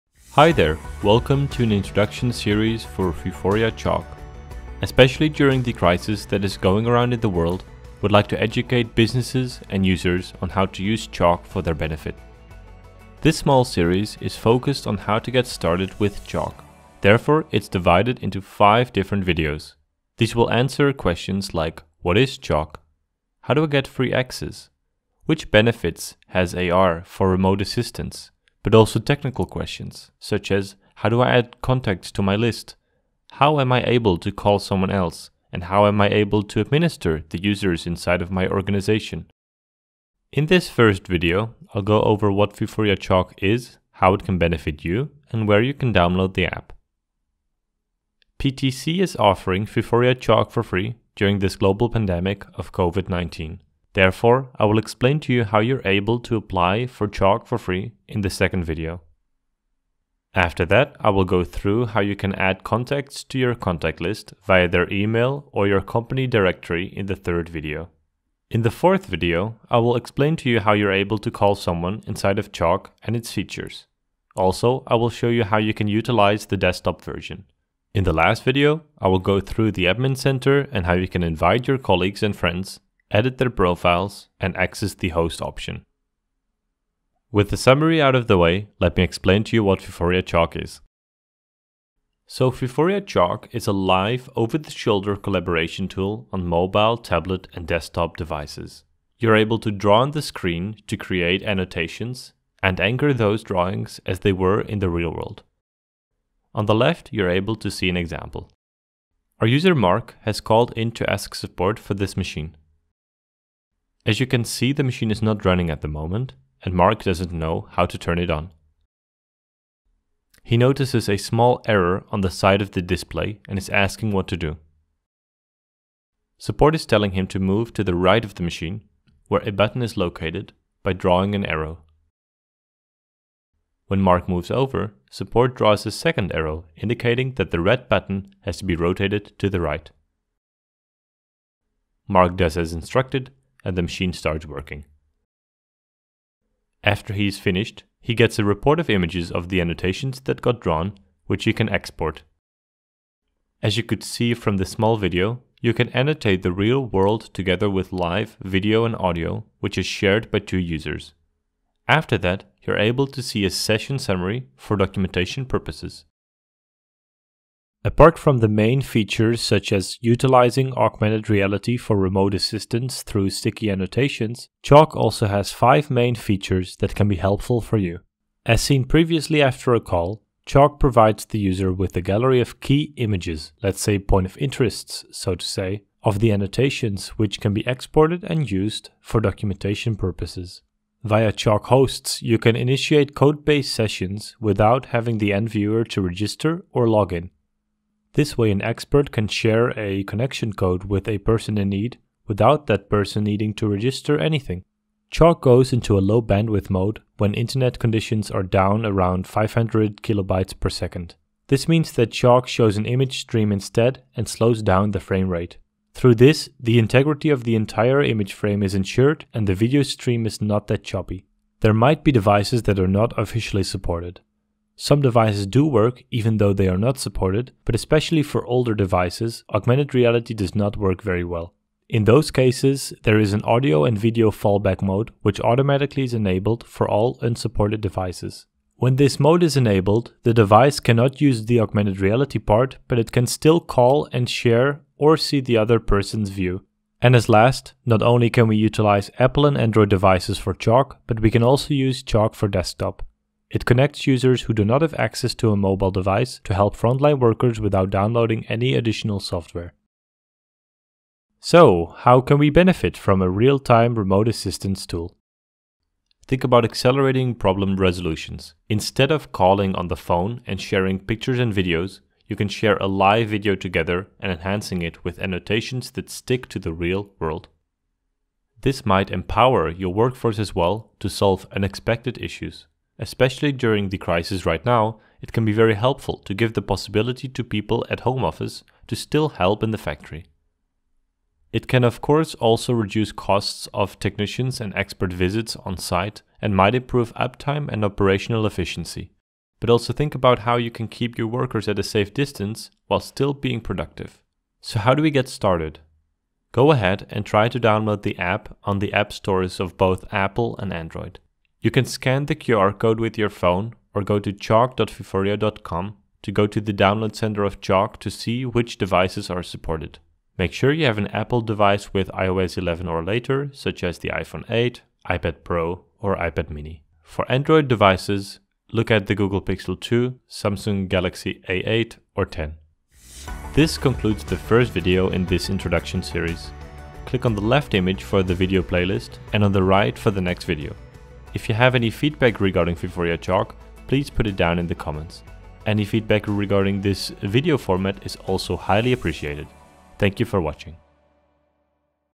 Vuforia Chalk - Introduction
In this first video we’ll kick-off a short introduction series for Vuforia Chalk by PTC. We’ll discuss what Vuforia Chalk is, its features, how you can benefit from it, and where to download it.

This introduction series provides you with the knowledge to start using Vuforia Chalk.